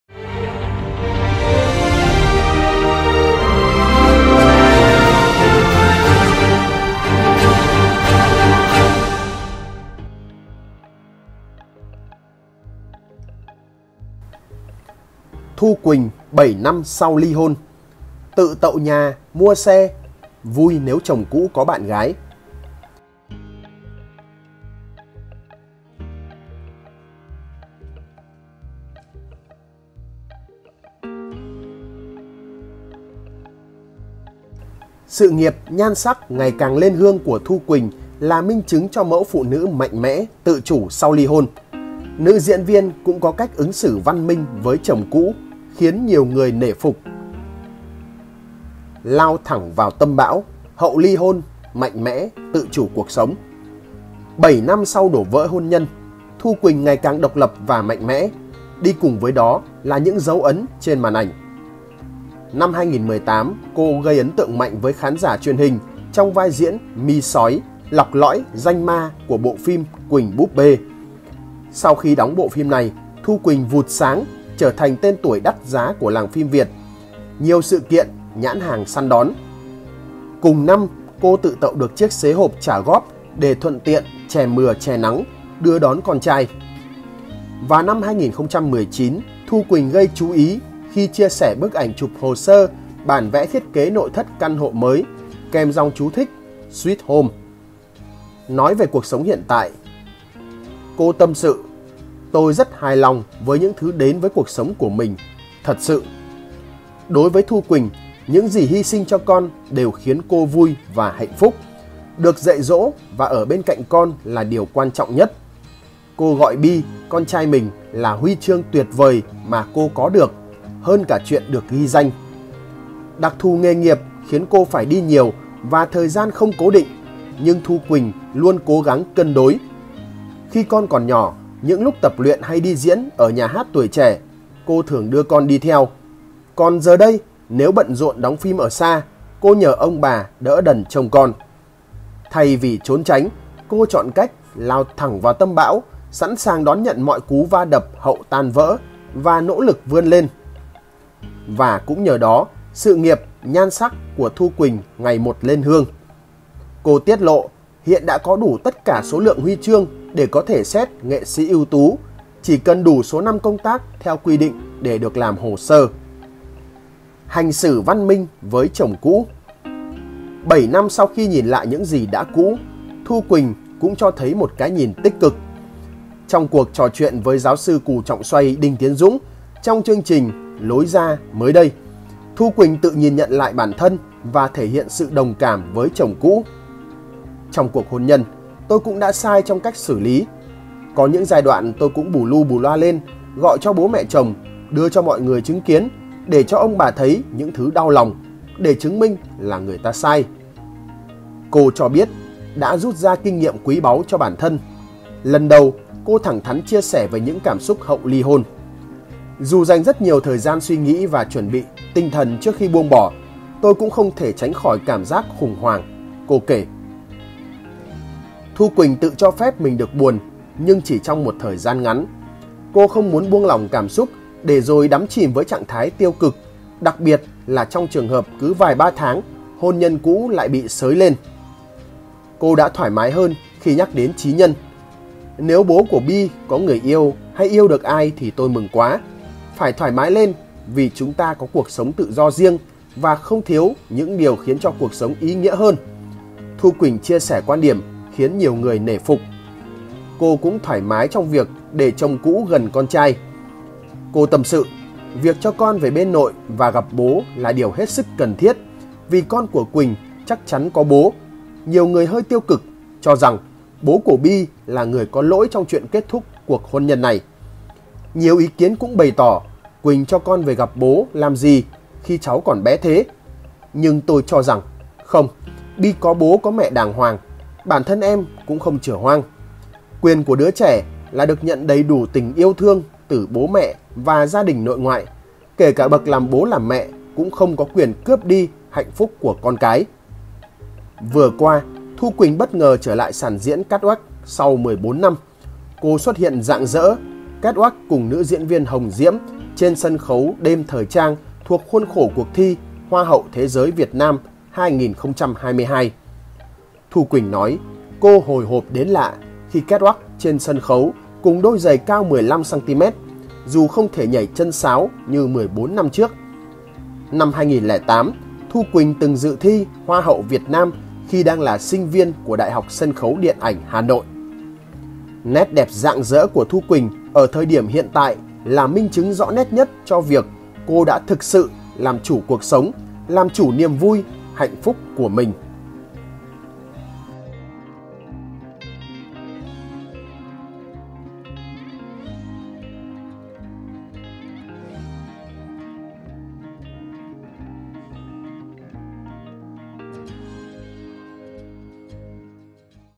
Diễn viên THU QUỲNH - 7 năm sau ly hôn Tự tậu nhà, mua xe, vui nếu chồng cũ có bạn gái
7 năm sau đổ vỡ hôn nhân, Thu Quỳnh ngày càng độc lập và mạnh mẽ, đi cùng với đó là những dấu ấn trên màn ảnh.
Năm 2018, Thu Quỳnh gây ấn tượng mạnh với khán giả truyền hình trong vai diễn "My sói" lọc lõi, ranh ma của bộ phim "Quỳnh búp bê". Sau khi đóng bộ phim này, Thu Quỳnh vụt sáng, trở thành tên tuổi đắt giá của làng phim Việt, nhiều sự kiện, nhãn hàng săn đón. Cùng năm, cô tự tậu được chiếc xế hộp trả góp để thuận tiện "che mưa che nắng", đưa đón con trai.
Vào năm 2019, Thu Quỳnh gây chú ý khi chia sẻ bức ảnh chụp hồ sơ bản vẽ thiết kế nội thất căn hộ mới tậu kèm dòng chú thích "sweet home" (tổ ấm ngọt ngào).
Nói về cuộc sống hiện tại, cô tâm sự với PV Dân trí: "Tôi rất hài lòng với những thứ đến với cuộc sống của mình, thật sự".
Đối với cô, những gì hy sinh cho con đều khiến cô vui và hạnh phúc. Được dạy dỗ và ở bên cạnh con là điều quan trọng nhất. Cô gọi Be - con trai mình là "huy chương" tuyệt vời mà cô có được, hơn cả chuyện được ghi danh.
Đặc thù nghề nghiệp khiến cô phải đi nhiều và thời gian không cố định nhưng Thu Quỳnh luôn cố gắng cân đối. Khi con còn nhỏ, những lúc tập luyện hay đi diễn ở Nhà hát Tuổi trẻ, cô thường đưa con đi theo. Còn giờ đây, nếu bận rộn đóng phim xa, cô nhờ ông bà đỡ đần trông con.
Thay vì trốn tránh, cô chọn cách "lao thẳng vào tâm bão", sẵn sàng đón nhận mọi cú va đập hậu tan vỡ và nỗ lực vươn lên. Và cũng nhờ đó, sự nghiệp, nhan sắc của Thu Quỳnh ngày một lên hương. Cô tiết lộ với PV Dân trí, hiện đã có đủ tất cả số lượng huy chương để có thể xét Nghệ sĩ Ưu tú, chỉ cần đủ số năm công tác theo quy định để được làm hồ sơ.
Hành xử văn minh với chồng cũ
7 năm sau khi nhìn lại những gì đã cũ, Thu Quỳnh cũng cho thấy một cái nhìn tích cực.  Trong cuộc trò chuyện với "Giáo sư Cù Trọng Xoay" Đinh Tiến Dũng trong chương trình Lối ra mới đây, Thu Quỳnh tự nhìn nhận lại bản thân và thể hiện sự đồng cảm với chồng cũ: "Trong cuộc hôn nhân, tôi cũng đã sai trong cách xử lý. Có những giai đoạn tôi cũng bù lu bù loa lên, gọi cho bố mẹ chồng, đưa cho mọi người chứng kiến, để cho ông bà thấy những thứ đau lòng. Để chứng minh là người ta sai…".
Cô cho biết, đã rút ra kinh nghiệm quý báu cho bản thân. Lần đầu, cô thẳng thắn chia sẻ về những cảm xúc hậu ly hôn. "Dù dành rất nhiều thời gian suy nghĩ và chuẩn bị tinh thần trước khi buông bỏ, tôi cũng không thể tránh khỏi cảm giác khủng hoảng", cô kể.